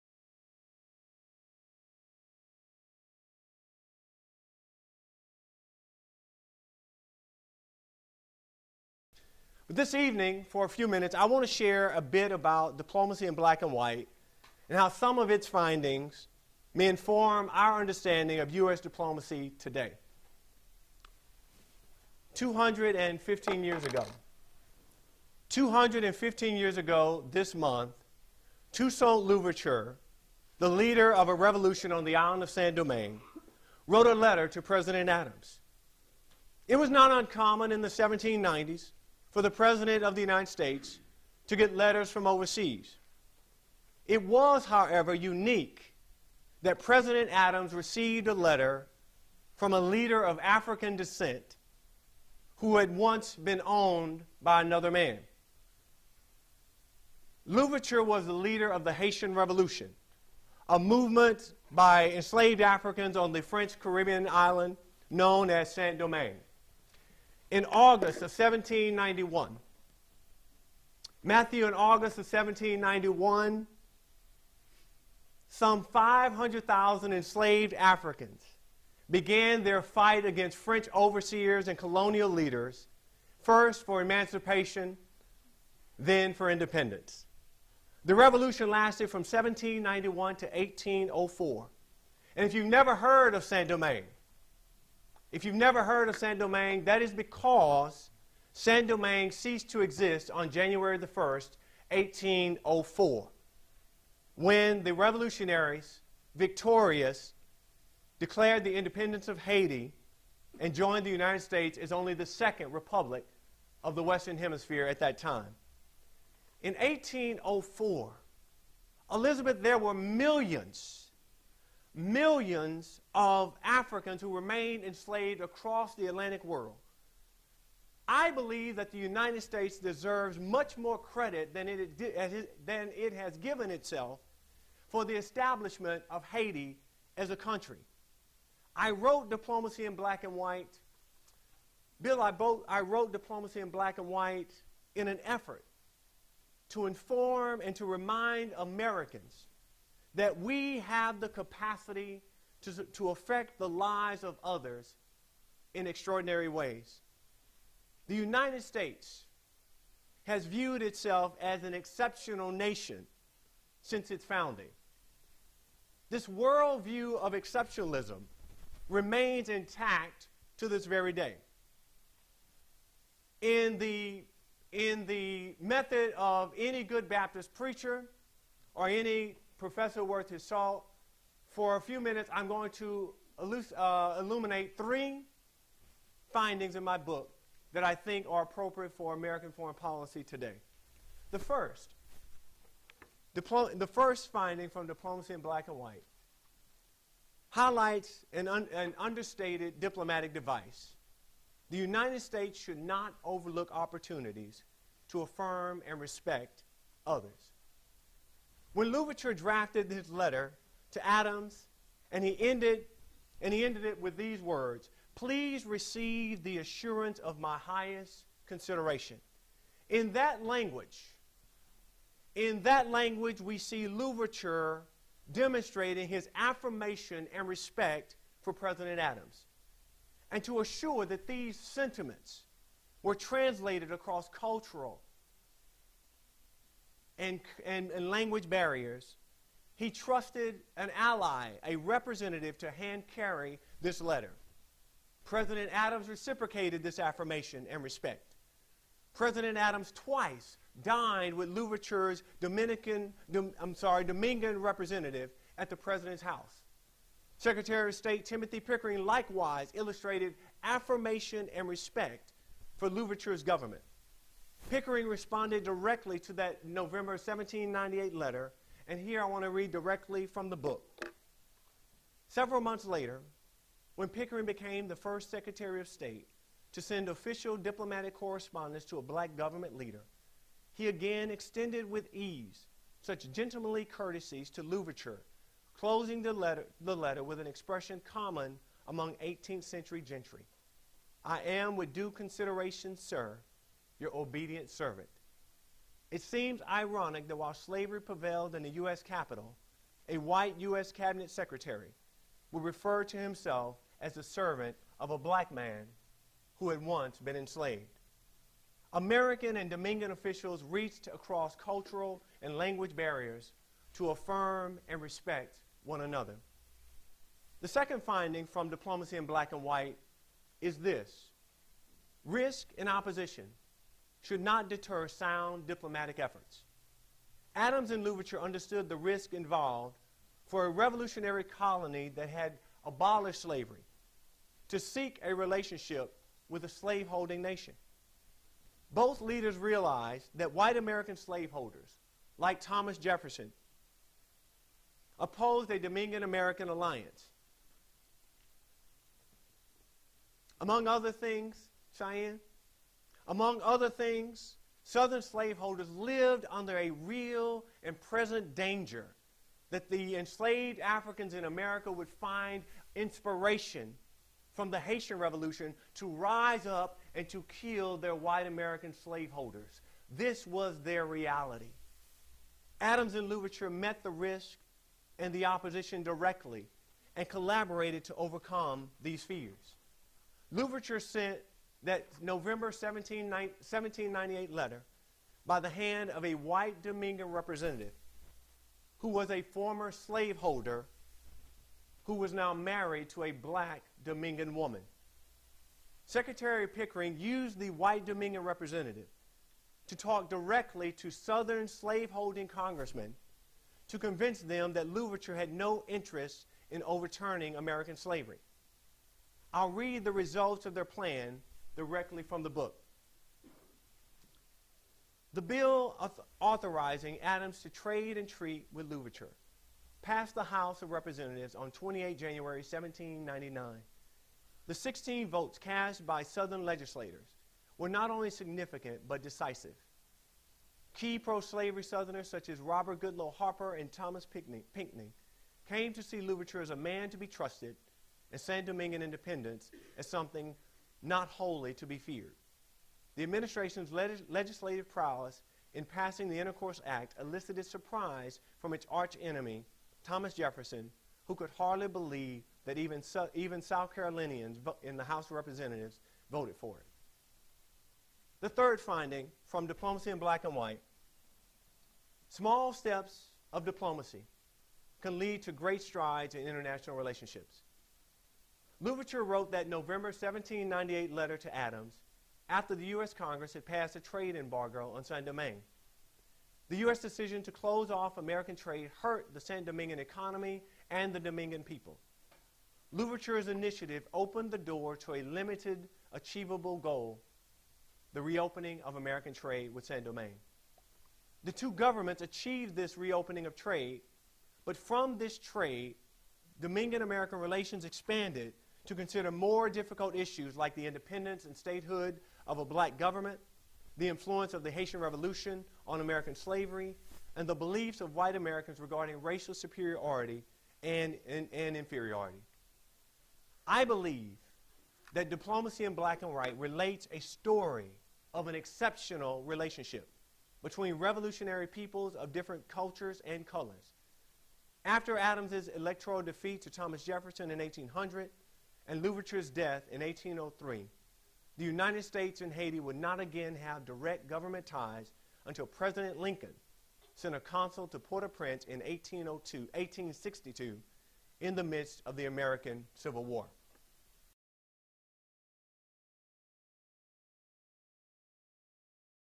Ronald Angelo Johnson on 'Diplomacy in Black and White'
Ronald Angelo Johnson gives speech "U.S. Diplomacy in the Age of Obama,"  using research findings from his book 'Diplomacy in Black and White."